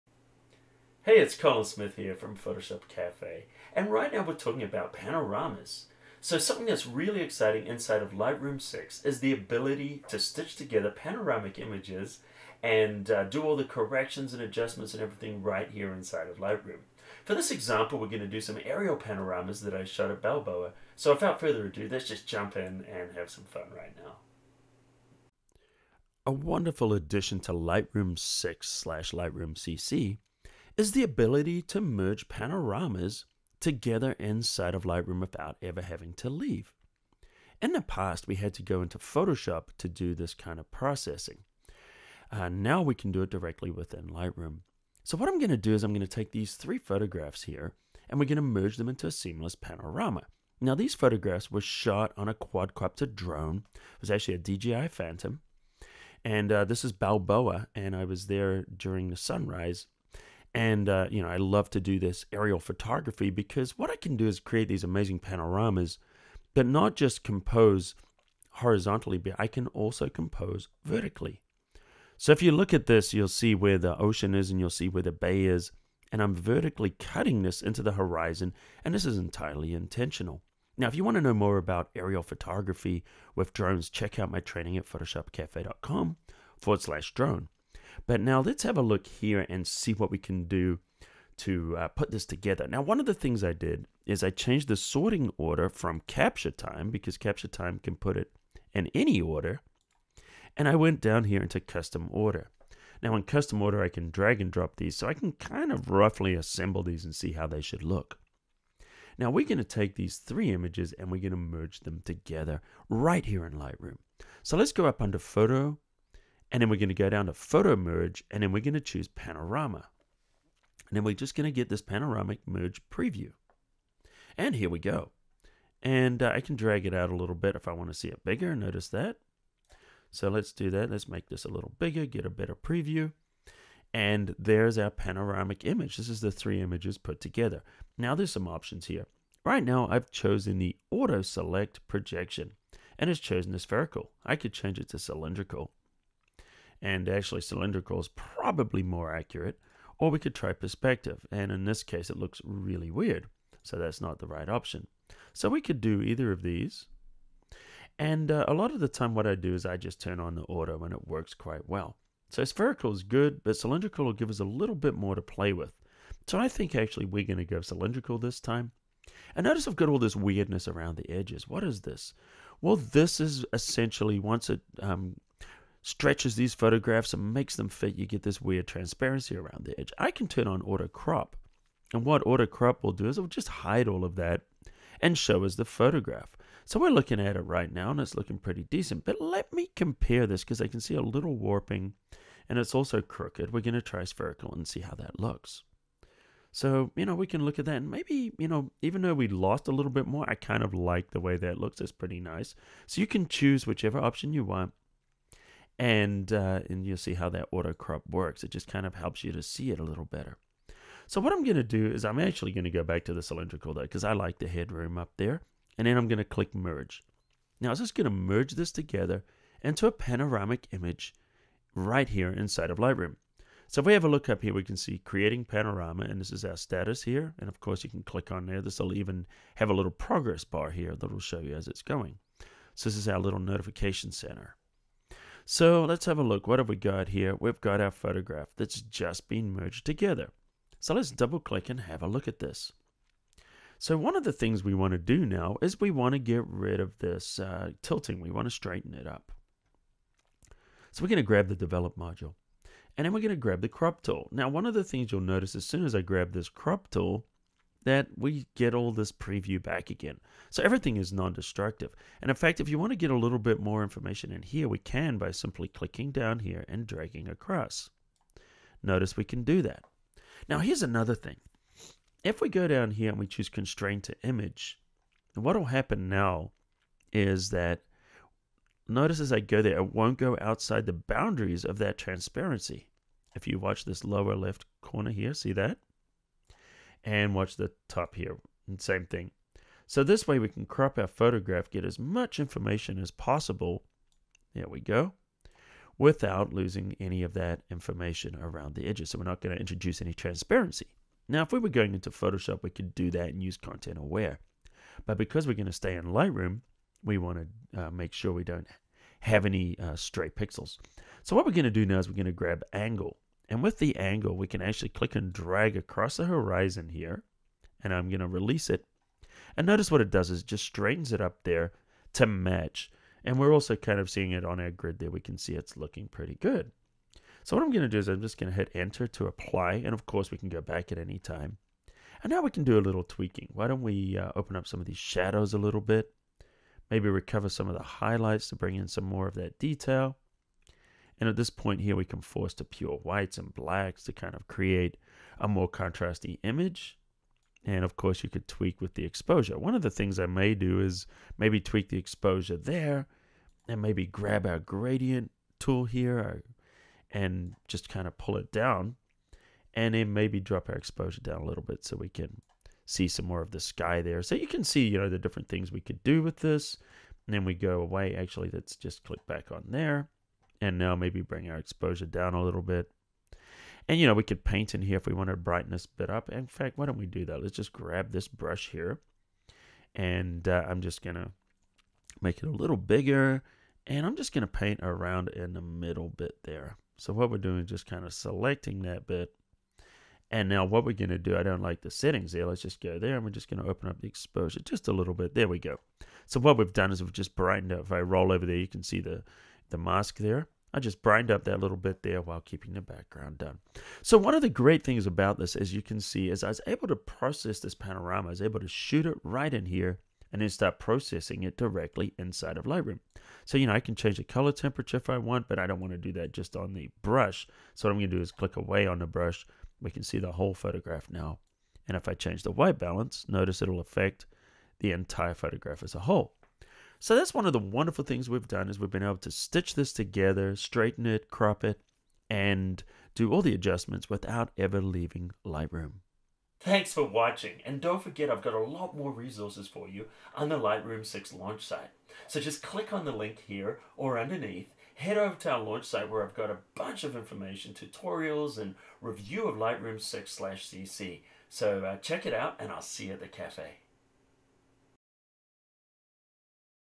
Lightroom tutorial, Merging Panoramas in Lightroom CC with Colin Smith
Learn how to stitch panoramas in Lightroom CC and Lightroom 6 tutorial. Colin Smith shows you how to make panoramic images in Lightroom CC using drone photos.. Full Lightroom 6 launch